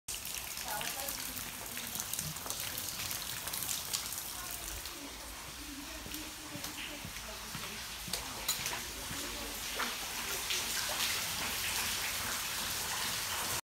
Flooding at Ardleigh Green Infants School
Flooding and structural damage has caused Ardleigh Green Infants School to close.